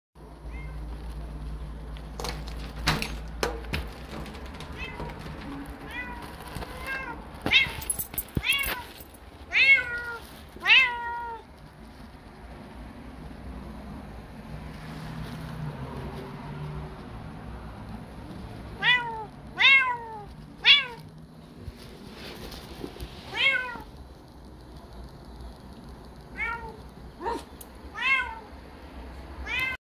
Cat Opens Door To Greet Owner
Hi everyone! Kokoa greets me every day when I come home from work! 🥰